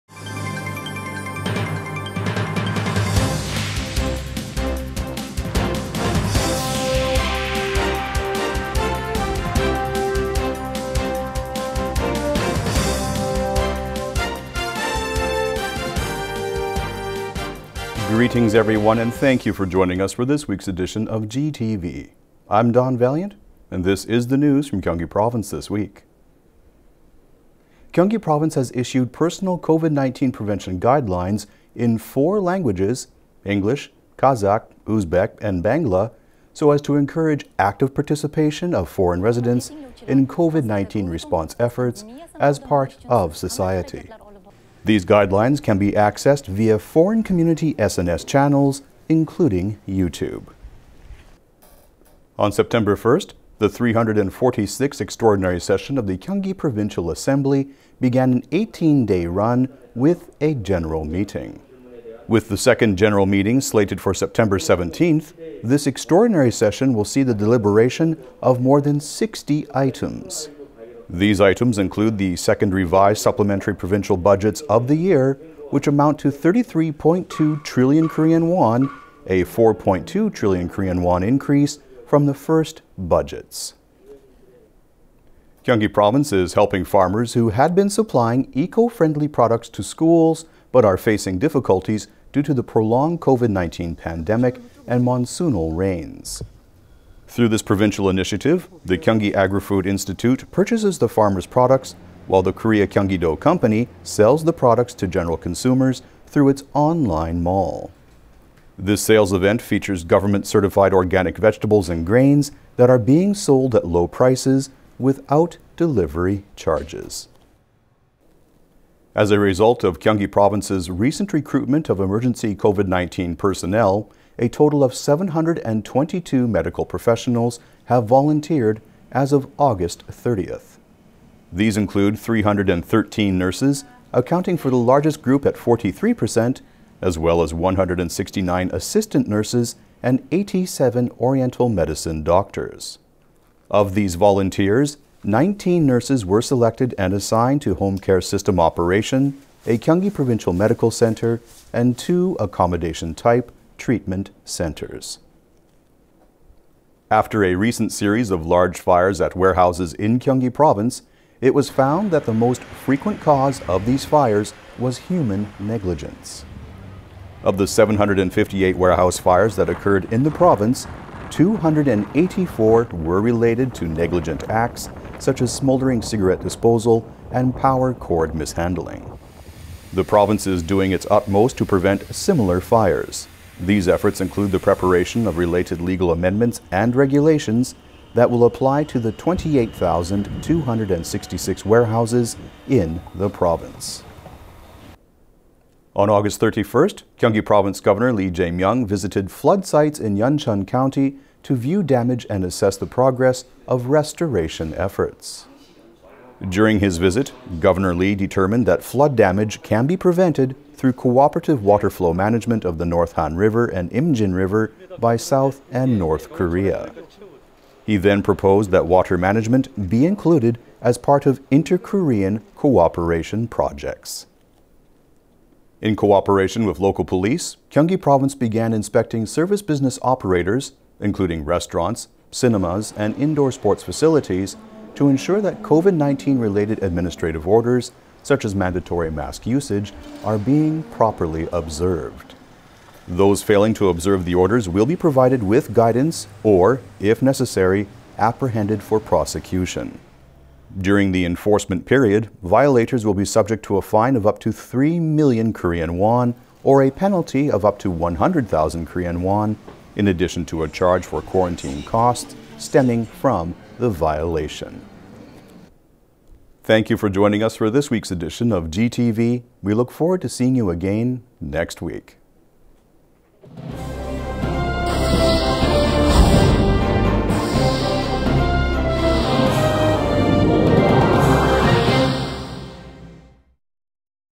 Gyeonggi Issues COVID-19 Guidelines in 4 Languages
1. Gyeonggi Issues COVID-19 Guidelines in 4 Languages
2. 346th Gyeonggi Provincial Assembly Extraordinary Session Begins
3. Gyeonggi Helps School Meal Food Suppliers Find Alternative Markets
4. Gyeonggi Province Emergency Medical Volunteers Begin Duties
5. Negligence Leading Cause of Warehouse Fires in Gyeonggi
6. Gyeonggi Governor Proposes Inter-Korean Water Management Cooperation
7. Gyeonggi Begins Checking Quarantine Order Observance at Business Sites